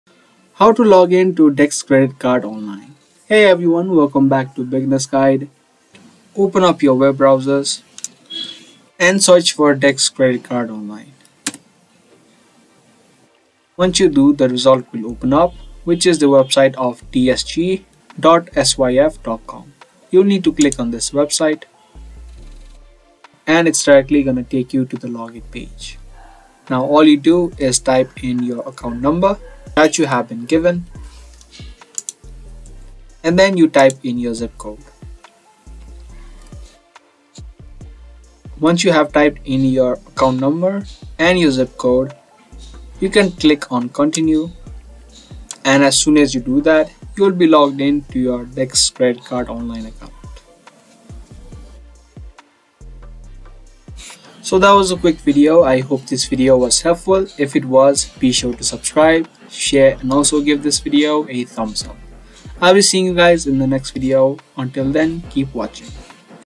Dick's Credit Card Login (2023) | How To Sign In To Dick's Credit Card Account Online (Step By Step)
In this video I'll be showing you how to login to your dick's credit card account. So watch the video till the end so that you won't miss any steps on how to log into dick's credit card account online in 2023.